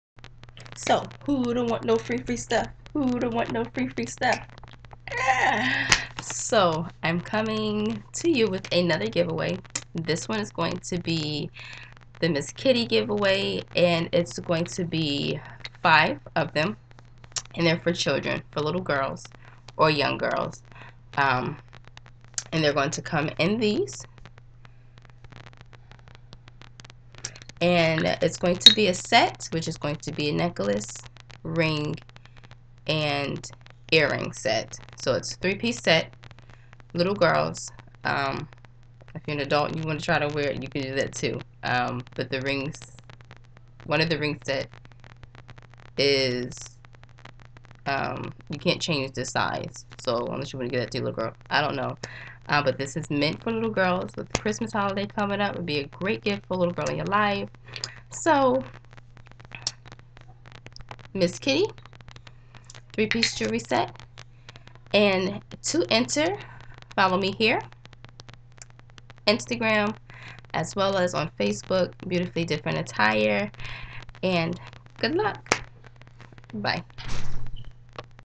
Miss Kitty Giveaway---- Little Girls Jewelry
Couldn't leave the little PrimaDonna's out so this Giveaway is for them!!!

Entry Rules:
Follow @beau_ti_fully_different on Instagram
Like our Facebook page Beautifully Different Attire

Good Luck!!!